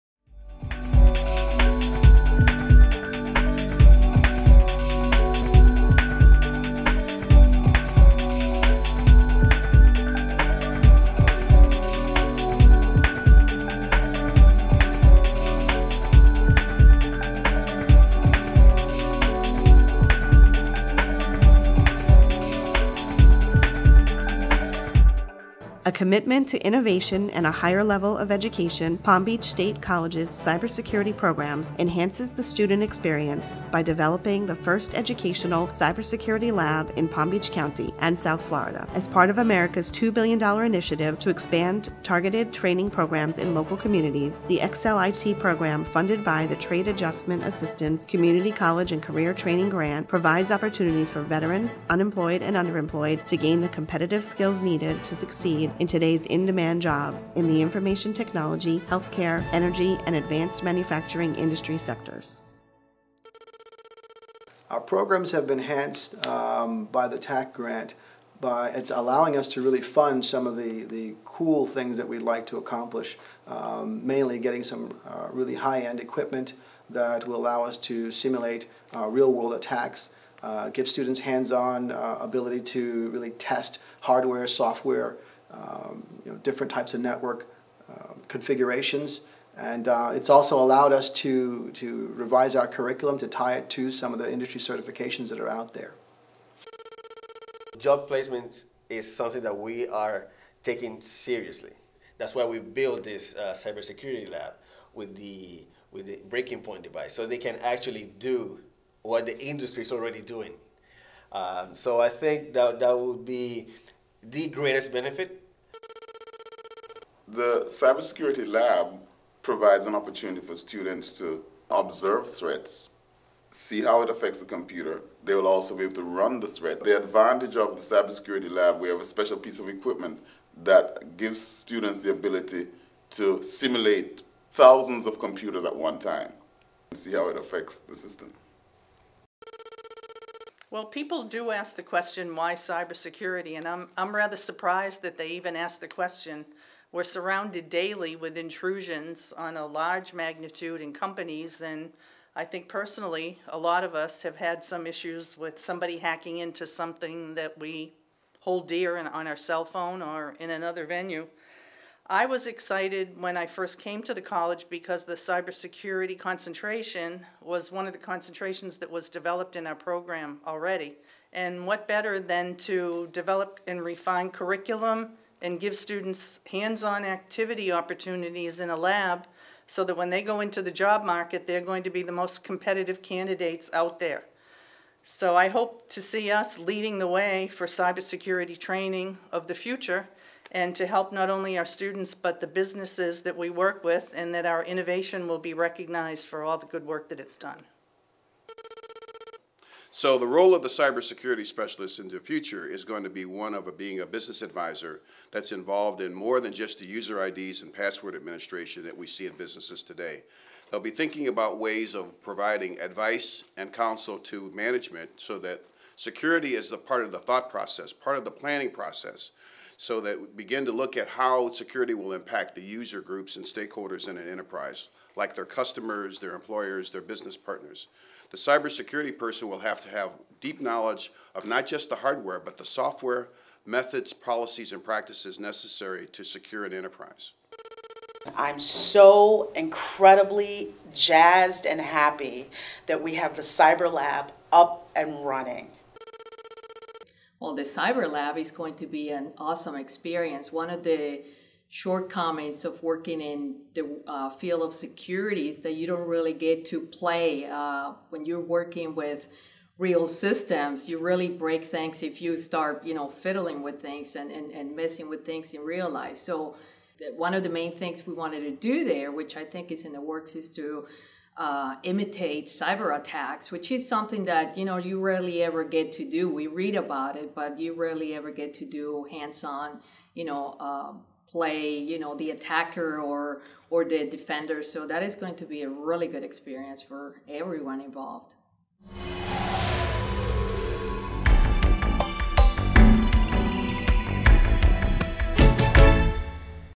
Inside PBSC’s Cybersecurity programs: Preparing students for careers battling hackers
Palm Beach State is preparing students for cybersecurity careers with industry-aligned degree and certificate programs, cyber-experienced faculty and a computer lab unlike any other in South Florida.